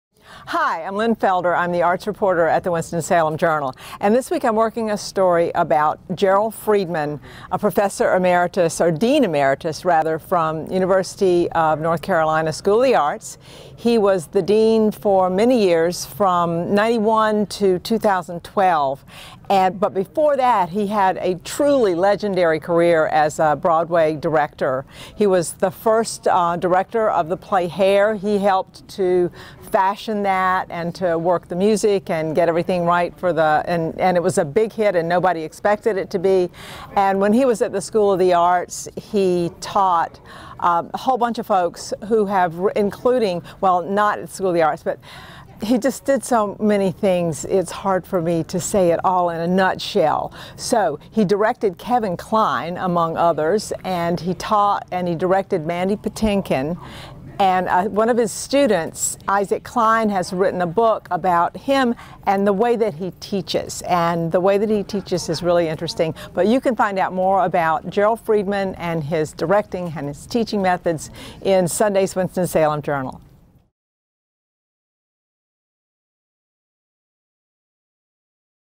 Coming Sunday: Legendary director Gerald Freedman
Arts reporter Lynn Felder looks at the career of Gerald Freedman, dean emeritus at the University of North Carolina School of the Arts​. He's directed Kevin Kline, Patti LuPone, Mandy Patinkin and actors you haven't heard of ... yet. Gerald Freedman, at 90, is the man of the hour. Now, there's a book about him and how he teaches and directs.